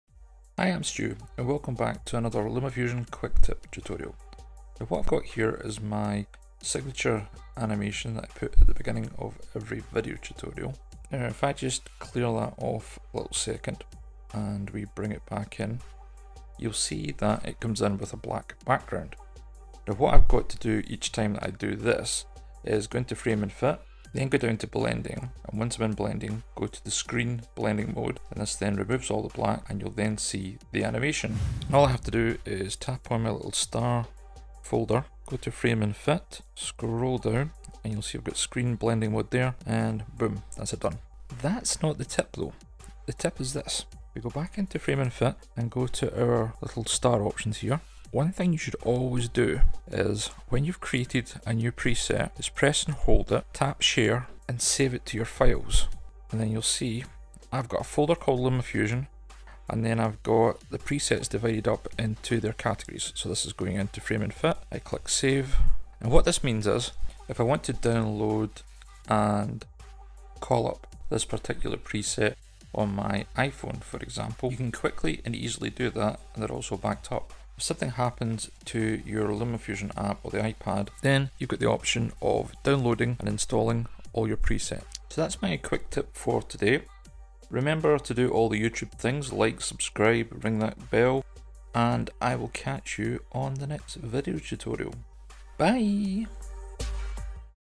Remember to Save and Back Up Your LumaFusion Editing Presets To The Files App
Whenever you create a preset within LumaFusion. You should get into the habit of immediately backing up by saving it into the cloud via the files app. Here’s a quicktip video tutorial showing you how I do it. 🤙

Stu

Tutorial #22